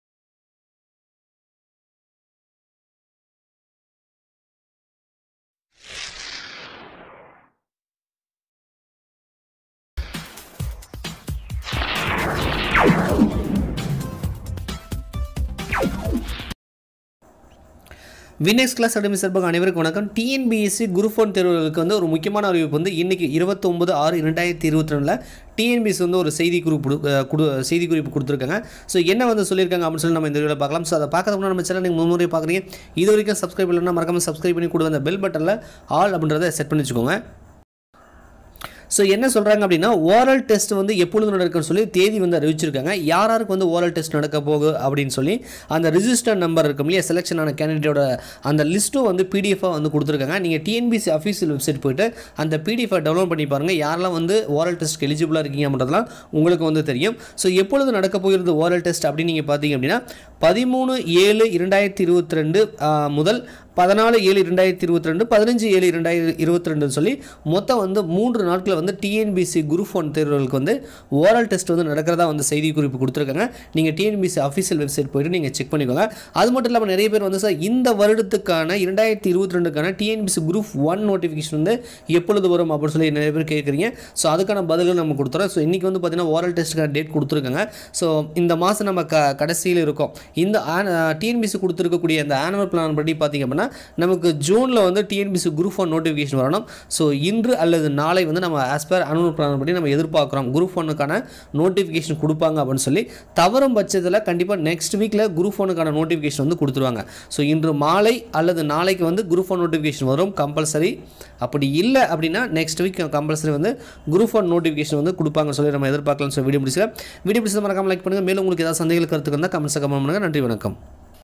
TNPSC GROUP 1 தேர்வு OT தேதி அறிவிப்பு / Notification 2022 எப்பொழுது?
Tnpsc group 1 தேர்வர்களுக்கு முக்கிய அறிவிப்பு

tnpsc group 1 oral test date out 13,14,15 july 2022

selection list & certificate verification

next tnpsc group 1 notification 2022 எப்பொழுது?